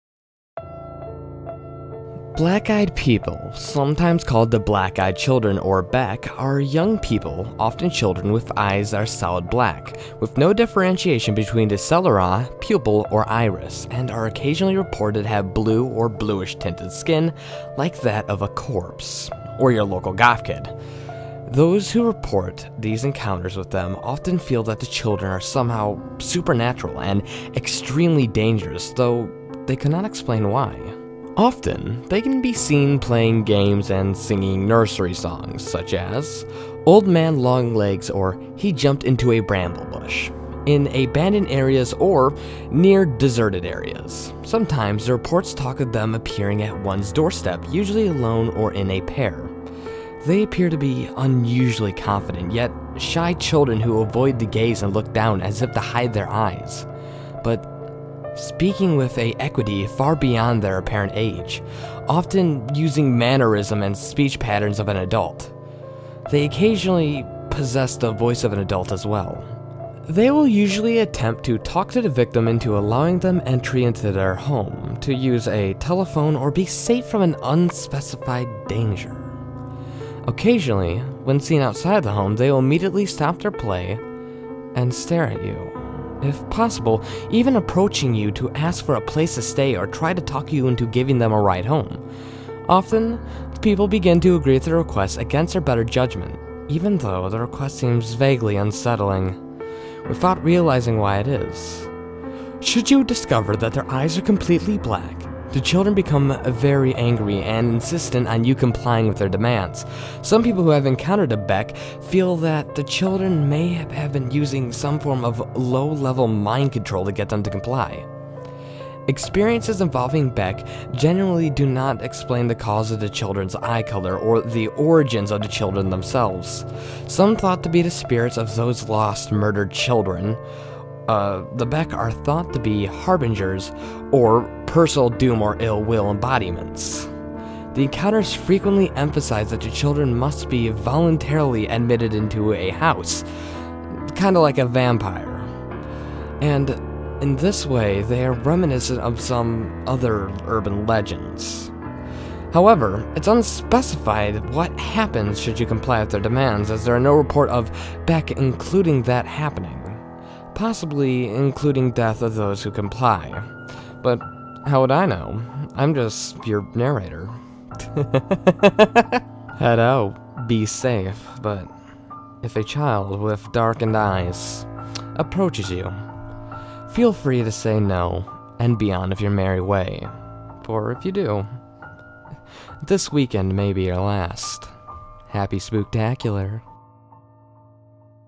"Black Eyed Children" Creepypasta
Hello my creepy watchers Id like to spin my eye across another story so sit down turn the lights off and prepare to be scared. Not to mention I have an Guest artist please check her out here!